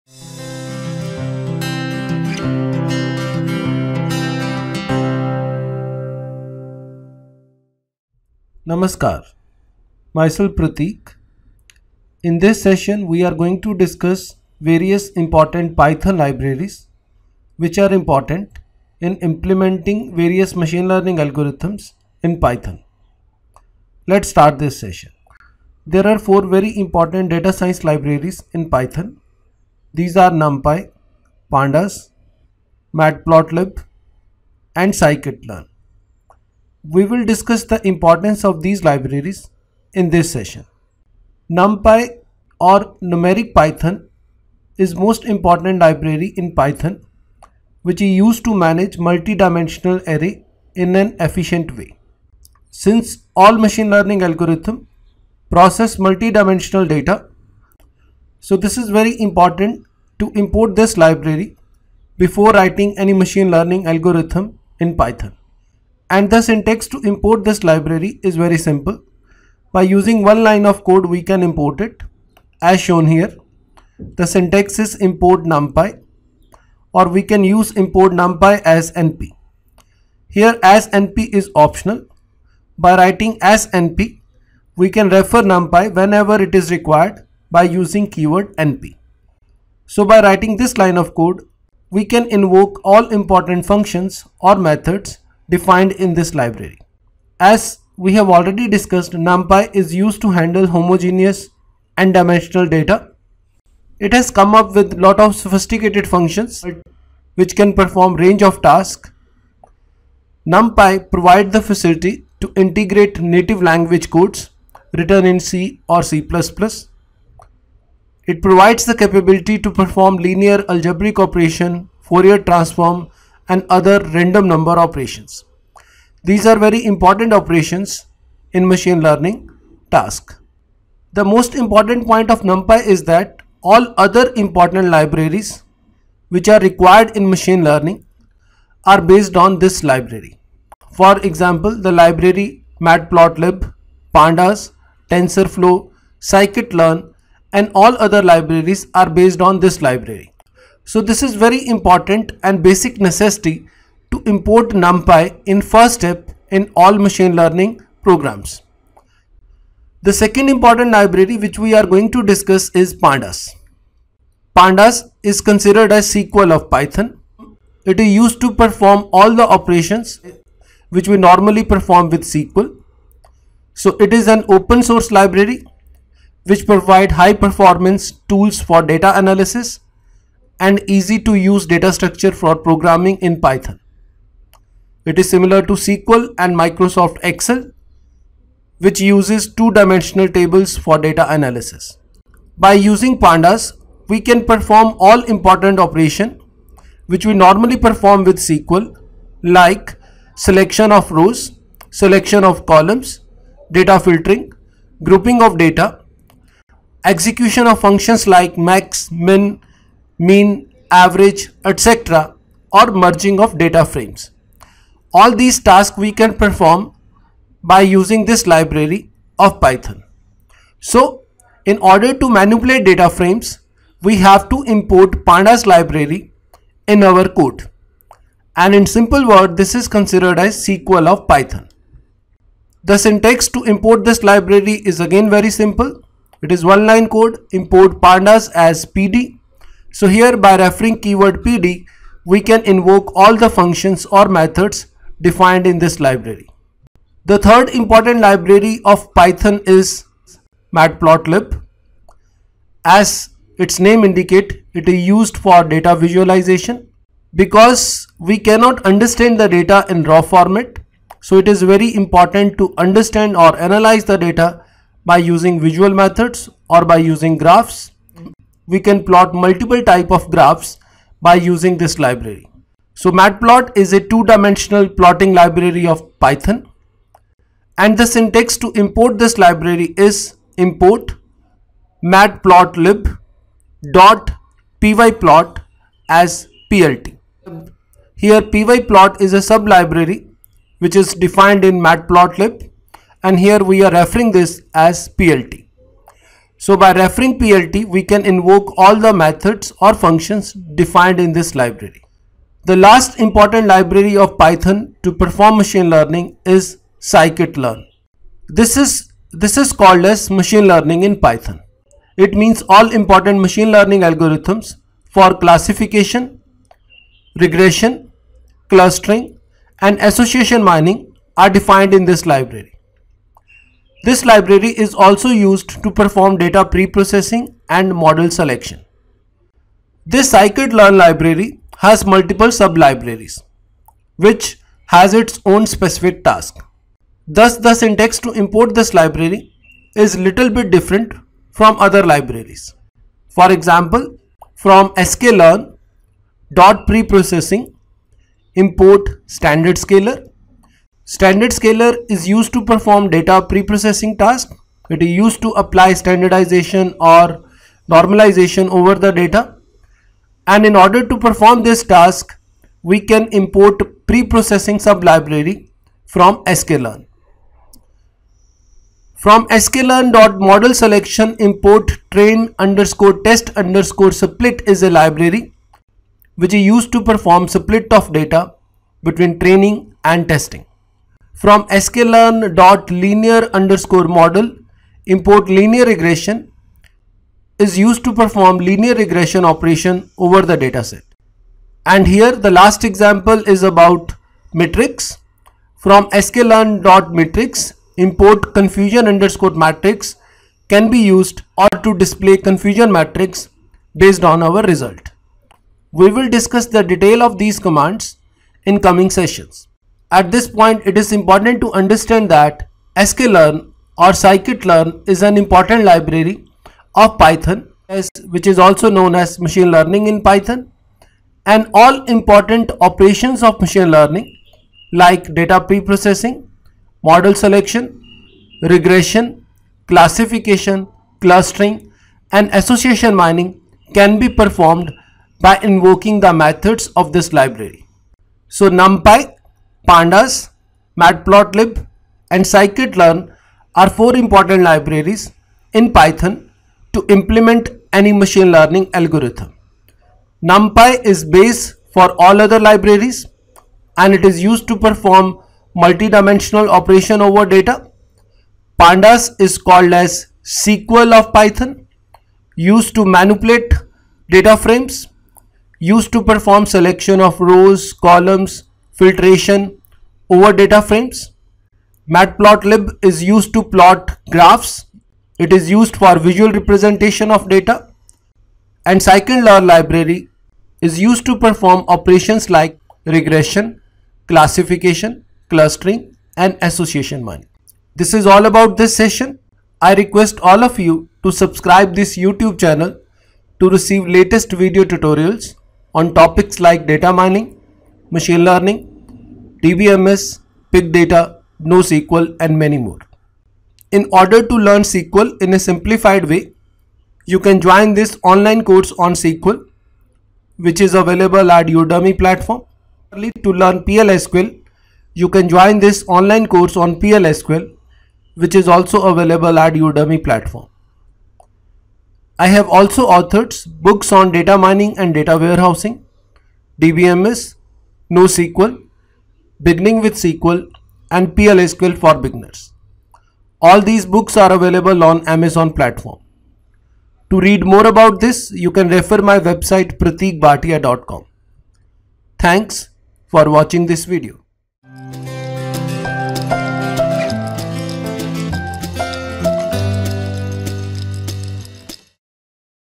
Python Libraries for Building Machine Learning Models | Parteek Bhatia | Data Mining
This video session discusses important Python Libraries like Numpy, Pandas, Matplotlib and Scikit-Learn for Building Machine Learning Models.